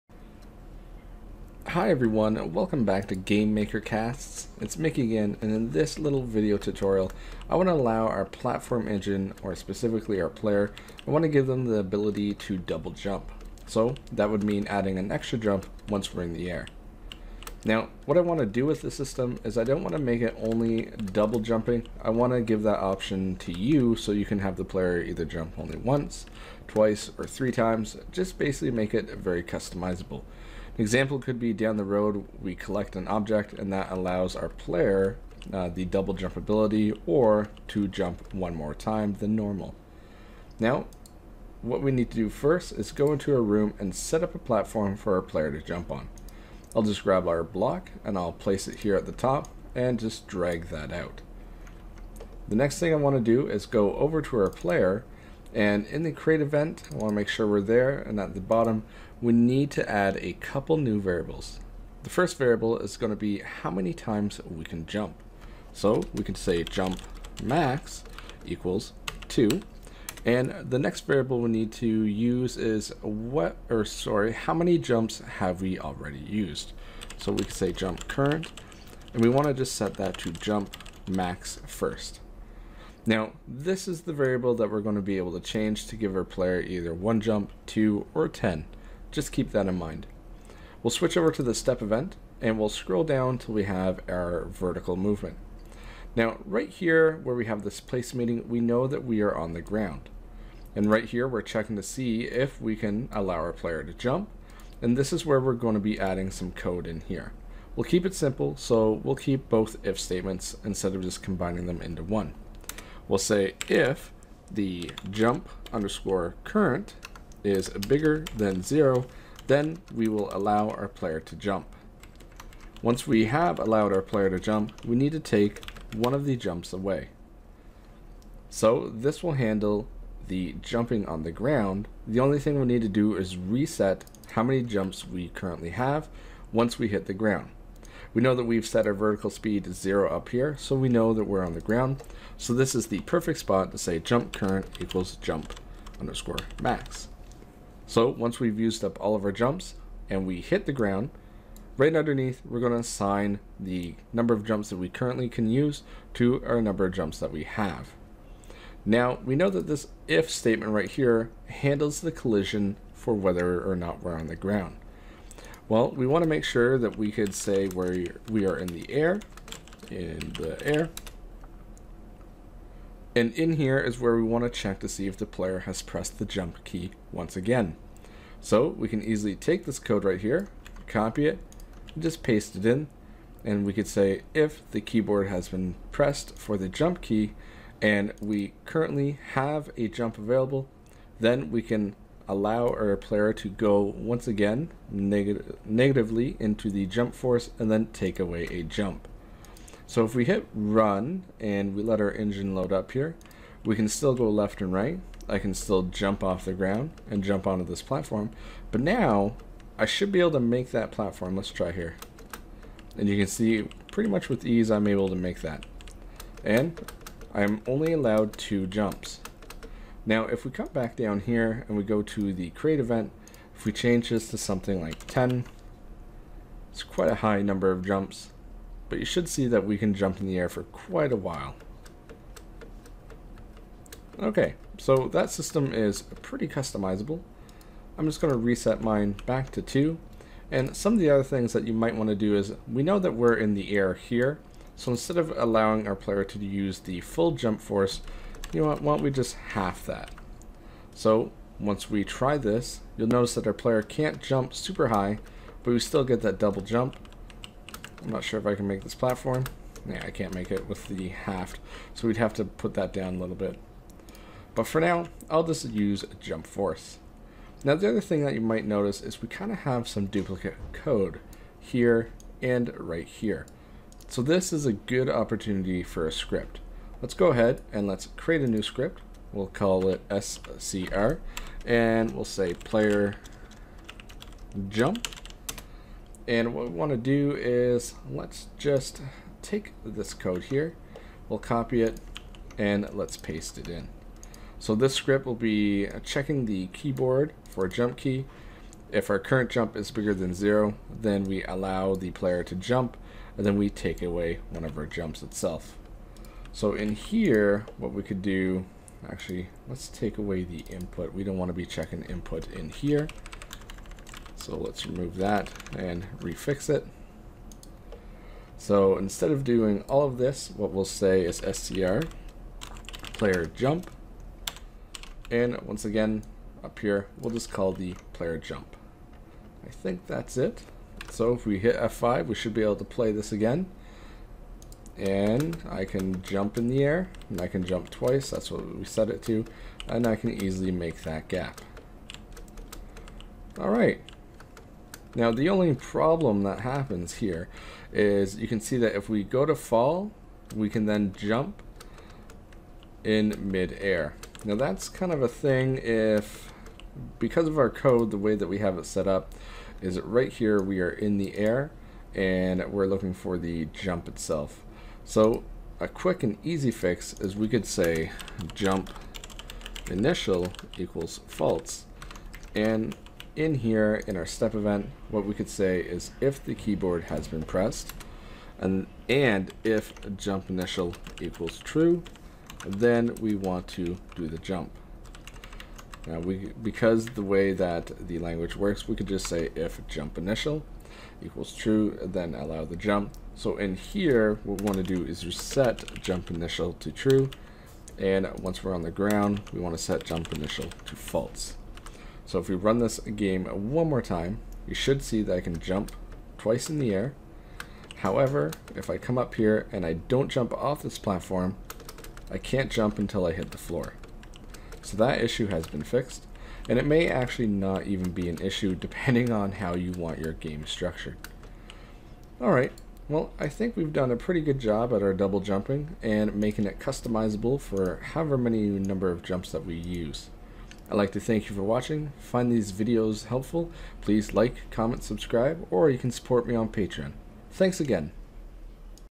Let's Platform - Lesson 2 - Double jumps!
Every once in awhile you will come across a game where you can jump in the air more than once.

Well, in this video we will be creating a system that will allow our player to jump twice, three or even four times in the air, 100% up to you and we can easily change it with one simple variable.